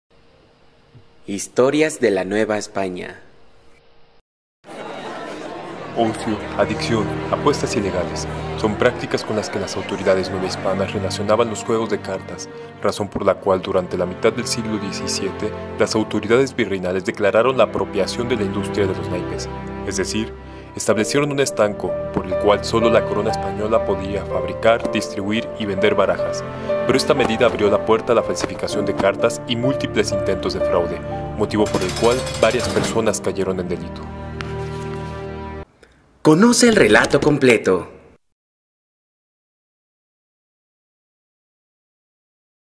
La falsificación de naipes un delito en la Nueva España
Los juegos de cartas en la época virreinal fueron una de las principales diversiones del pueblo novohispano, no obstante, su práctica intentó ser prohibida por las autoridades quienes consideraban que era un juego que fomentaba el vicio y las malas prácticas como las apuestas ilegales, pero a pesar de sus intentos de prohibición la gente continuaba adquiriendo naipes, incluso si se arriesgaban a comprar versiones falsas.